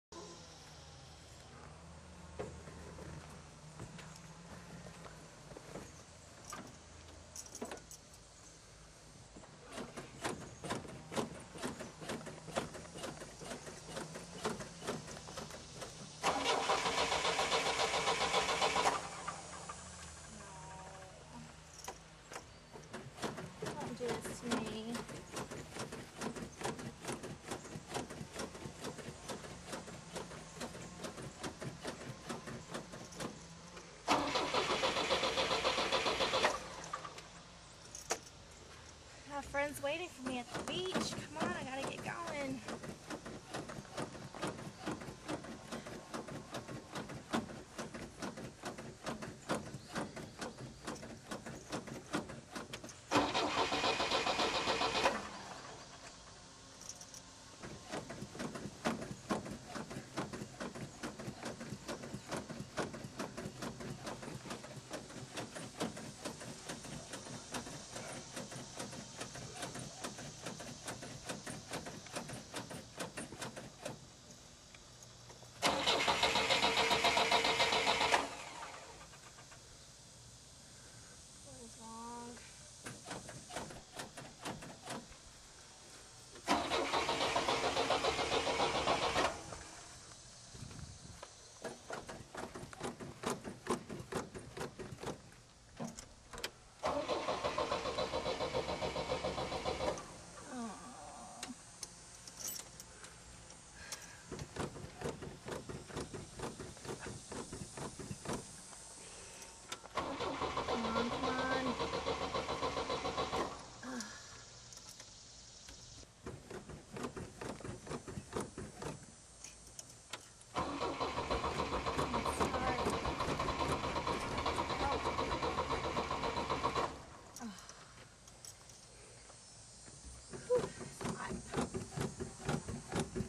Asian Kelly Bikini
Kelly wants to go to the pool but she can't get her 1962 Tempest convertible started.  Kelly now lives in Finland and has a public Vlog on Facebook under the name Kelly Keodara.